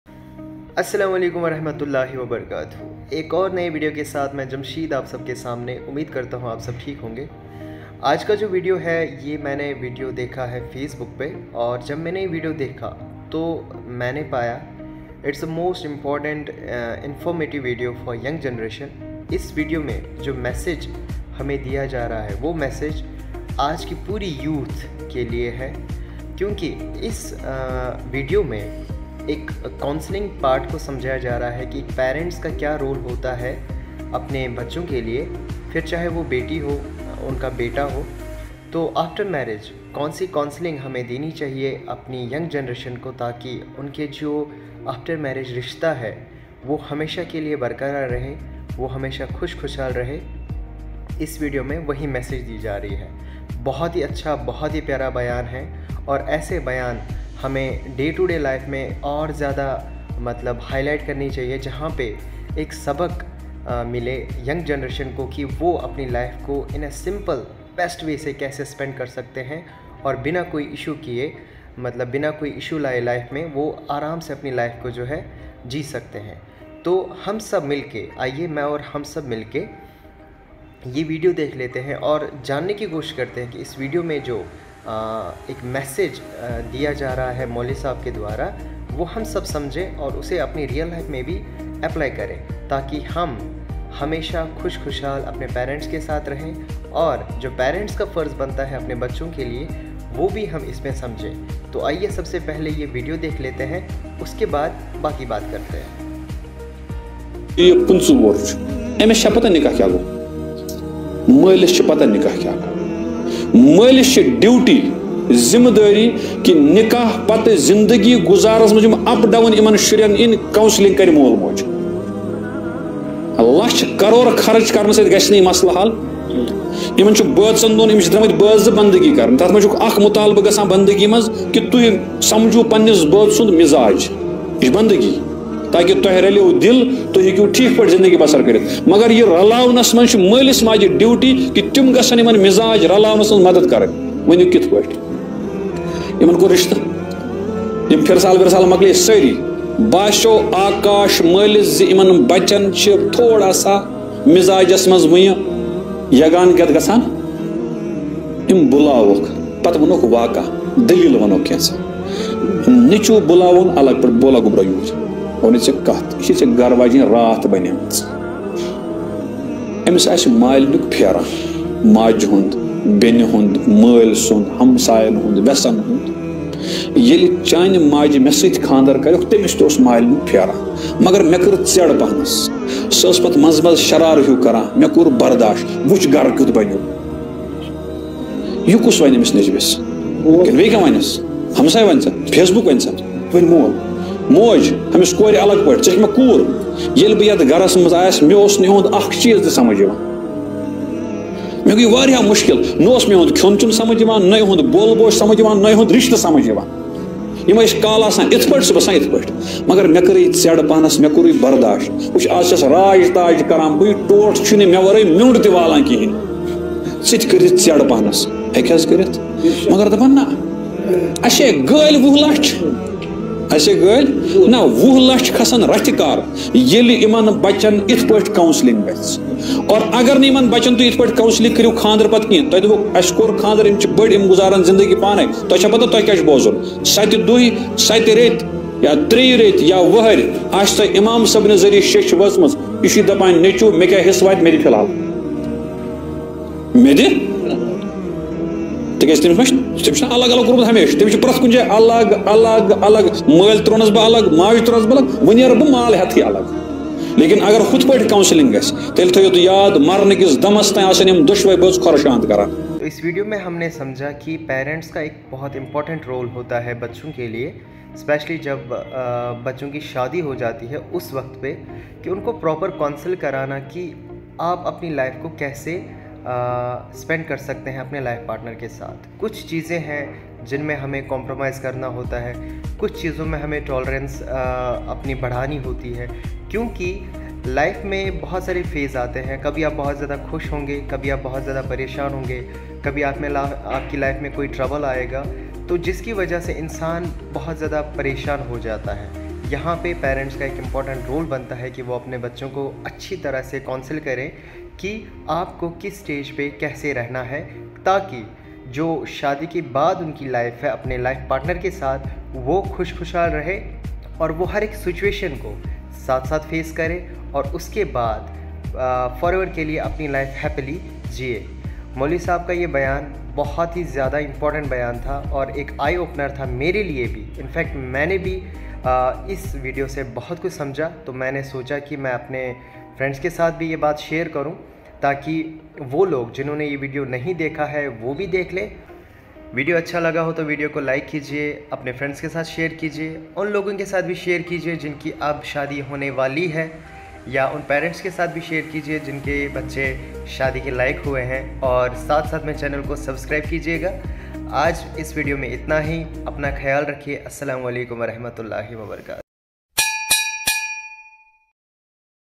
Role of Parents in counseling young Generation for a better Life. @mashayikh-ul-ummat
Every Parent, Married Persons and Unmarried Youngsters Must Watch it.
This Man Crushed My Heart into Pieces. No School or College had this in their Syllabus.